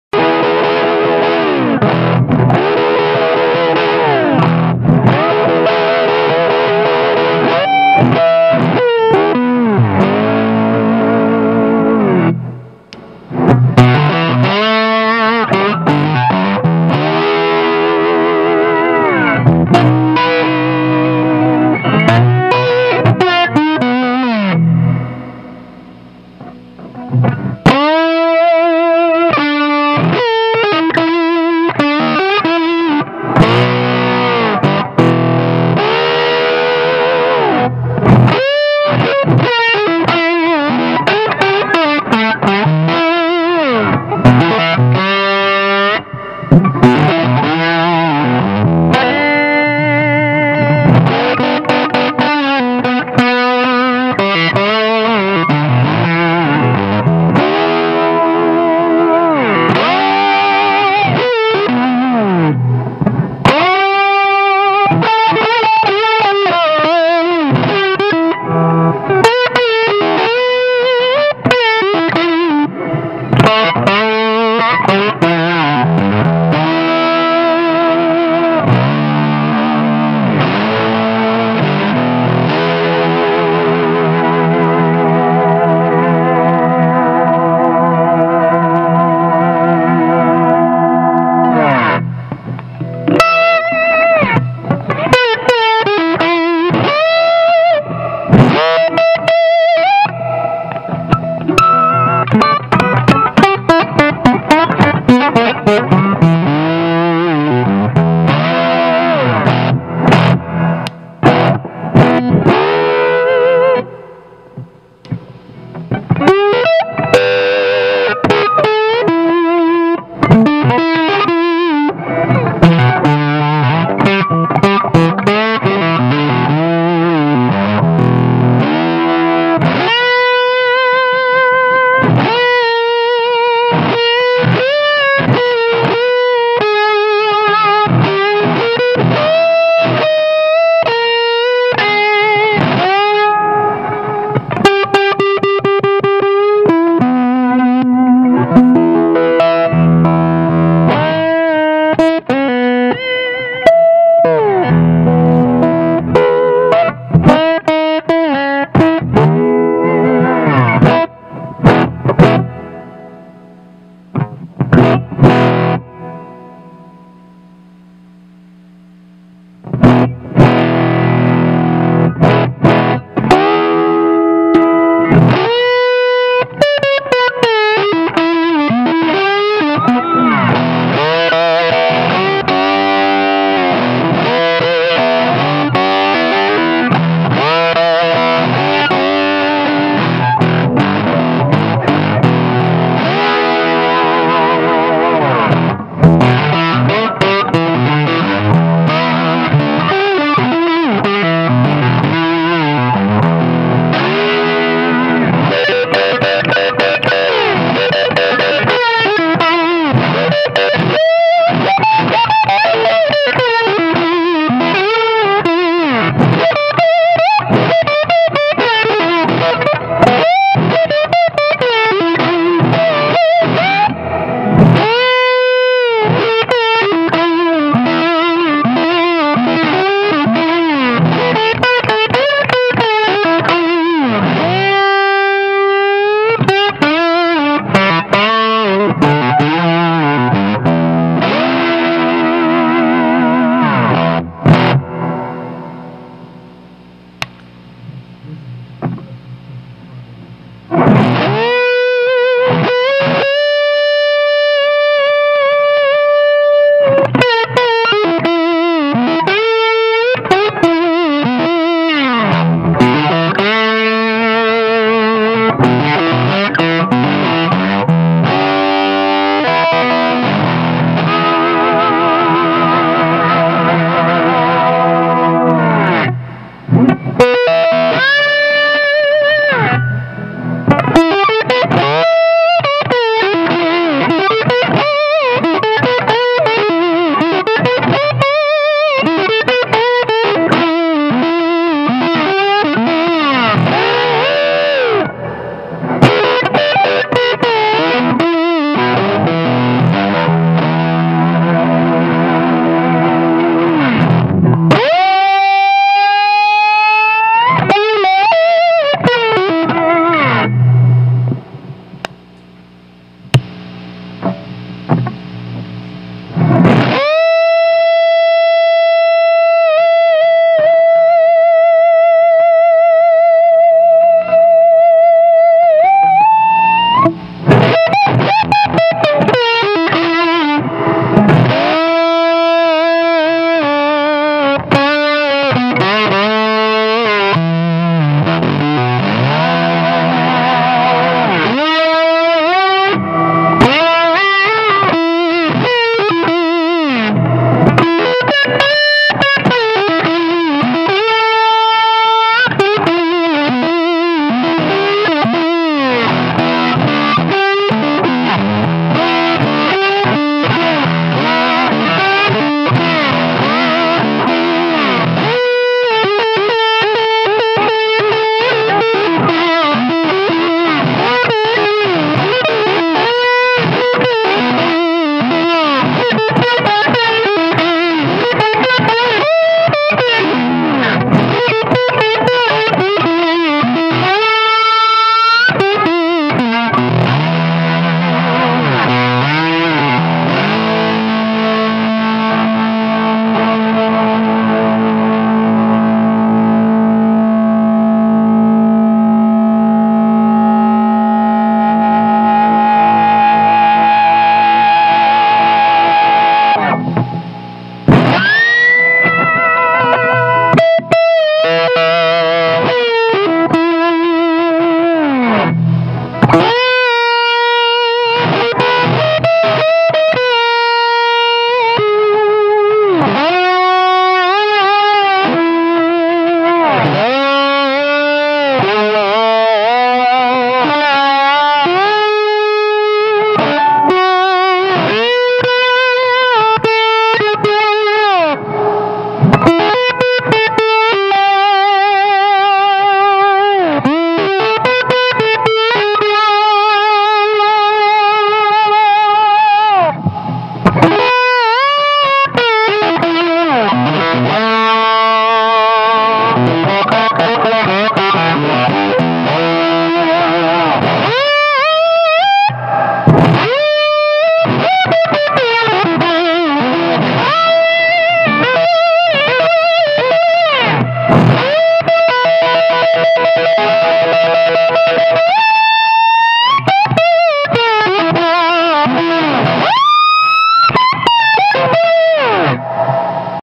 Soul Tramp Wraith Demo
Jeremy Montgomery playing slide on a Wraith